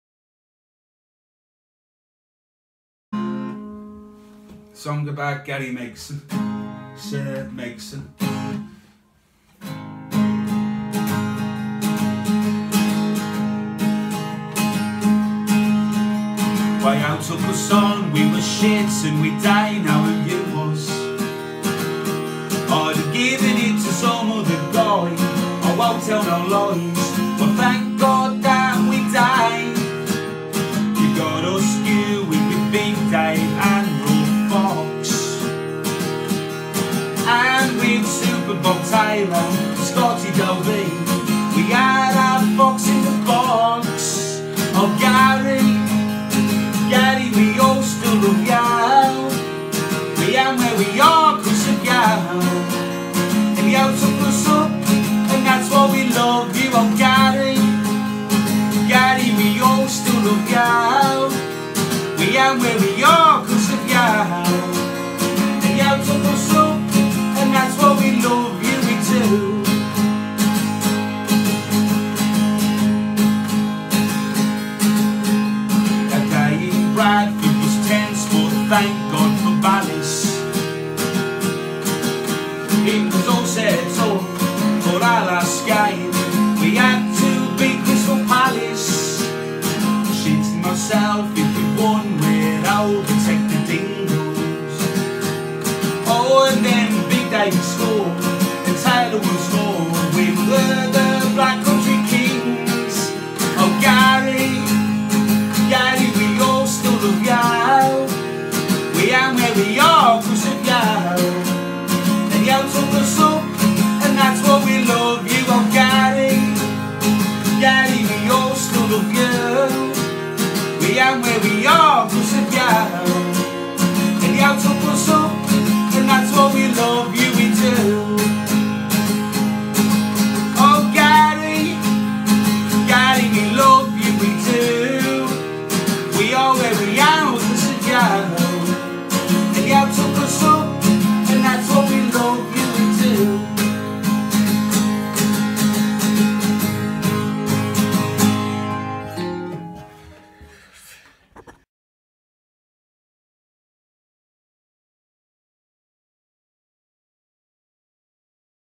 A song for Gary Megson
Sent by Tom Greenhalgh.
A song for Gary Megson, from an Albion fan.